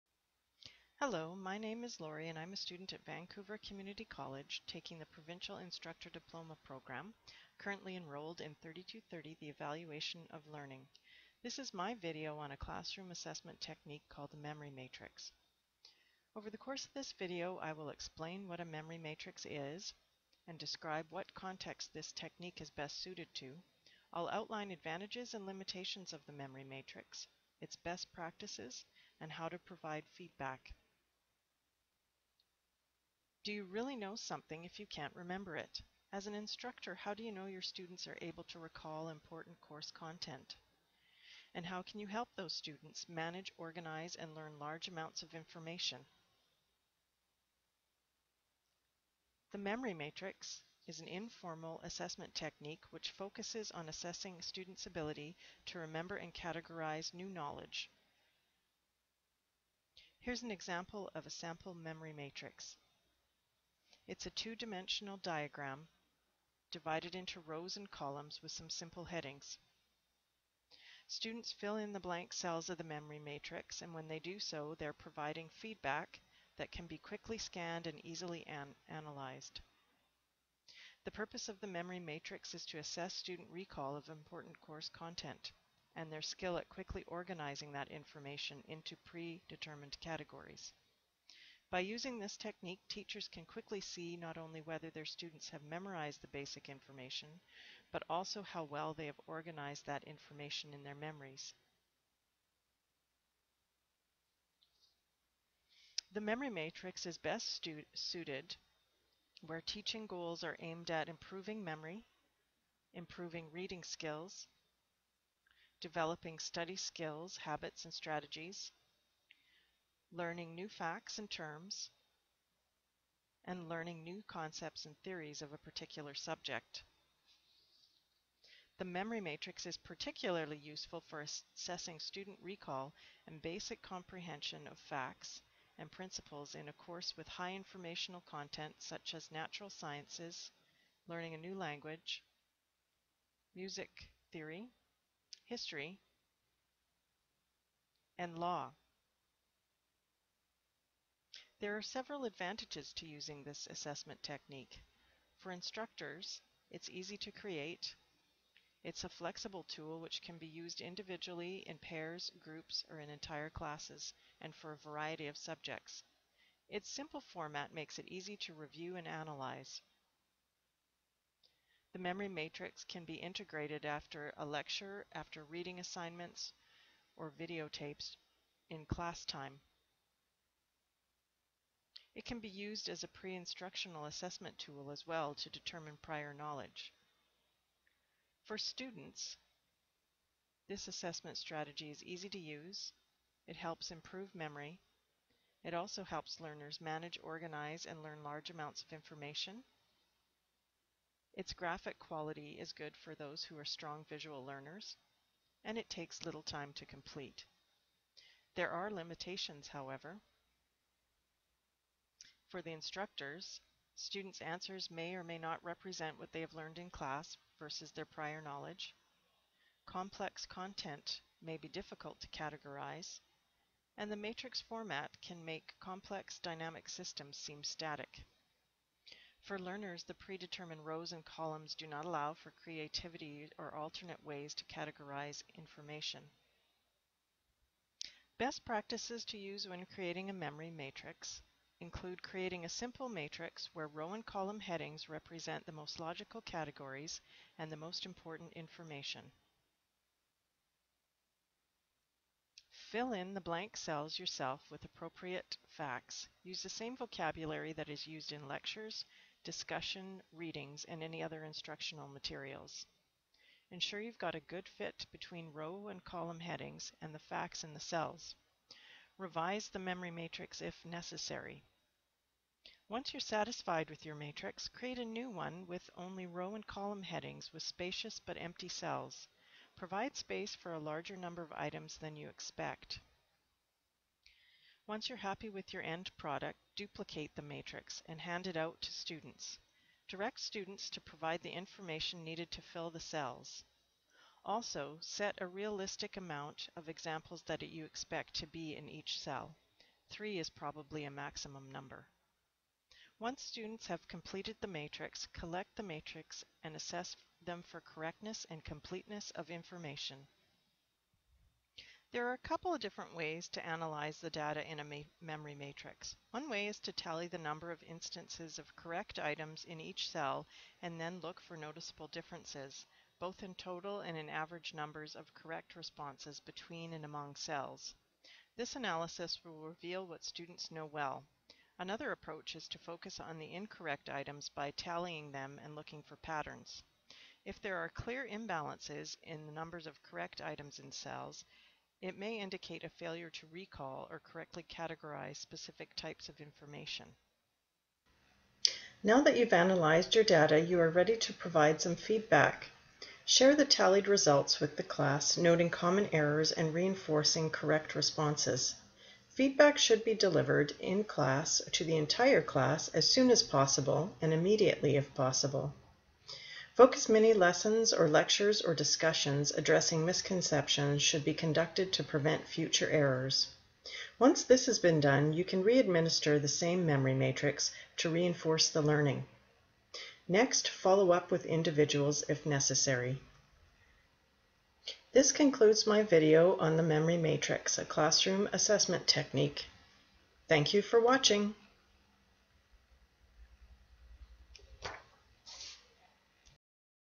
Memory Matrix: A Classroom Assessment Technique
This is a video describing what a Memory Matrix is, the best context to use it in, advantages and limitations of using it, best practices for creating your own Memory Matrix, and how to provide feedback using the data collected on a  Memory Matrix.